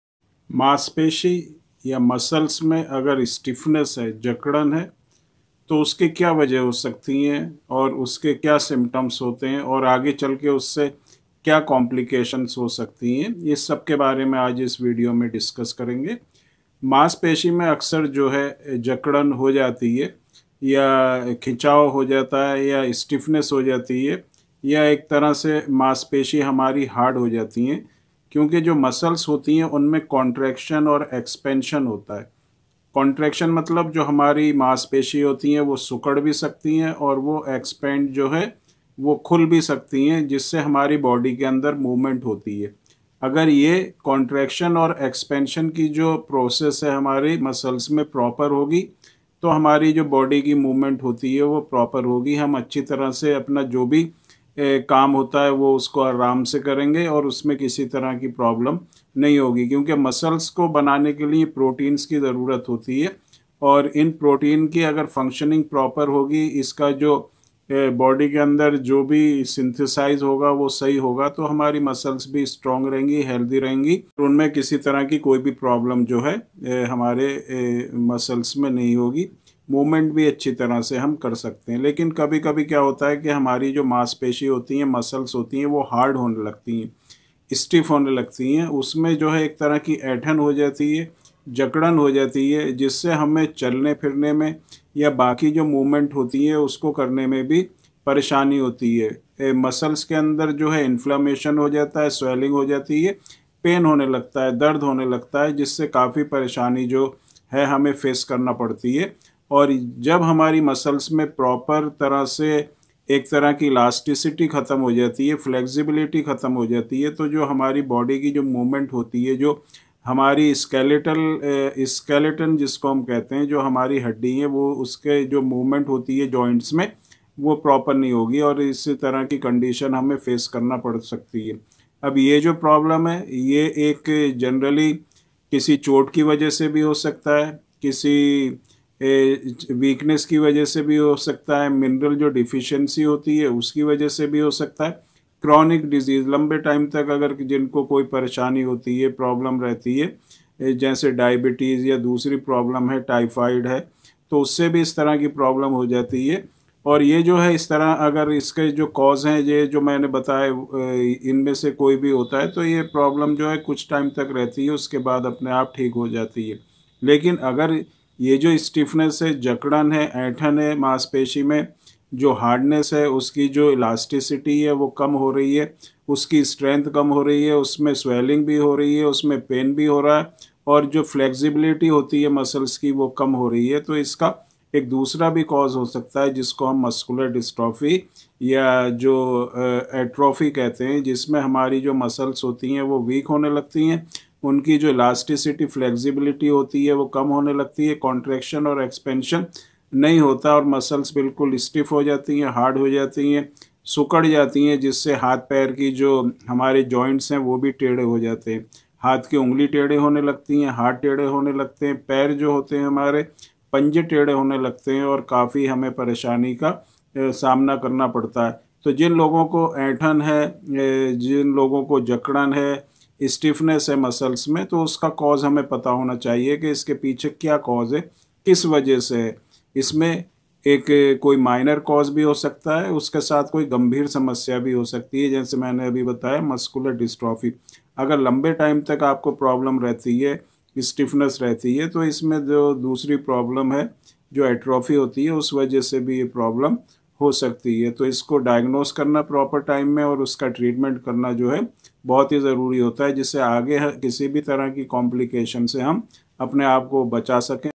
मस्कुलर डिस्ट्रॉफी में मांसपेशी बन जाती है चर्बी ! मसल्स में ताक़त कैसे लाएं
Dr Ashfaque khan phd